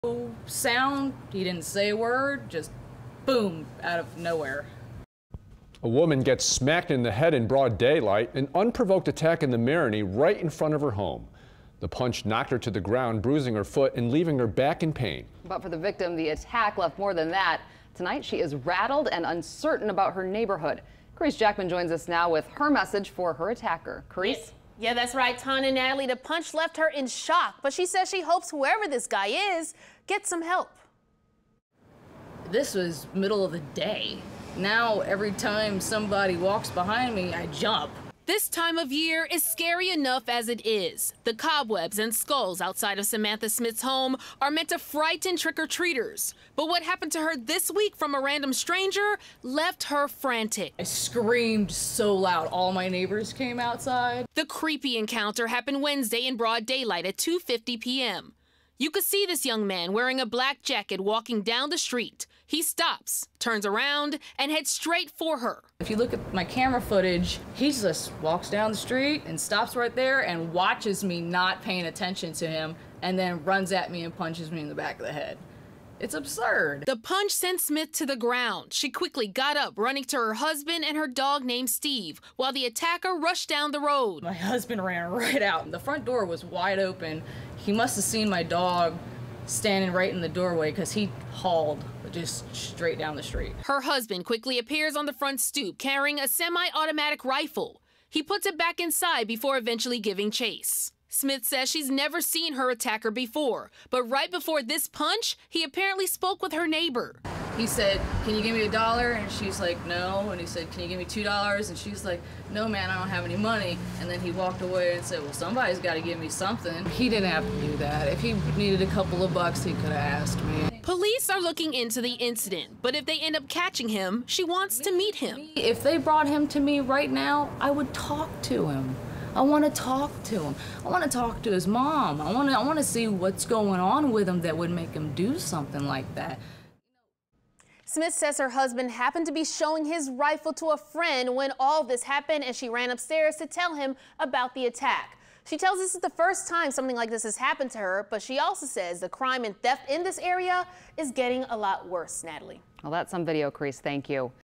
Woman smacked in head in the Marigny
She wants to speak to the boy who attacked her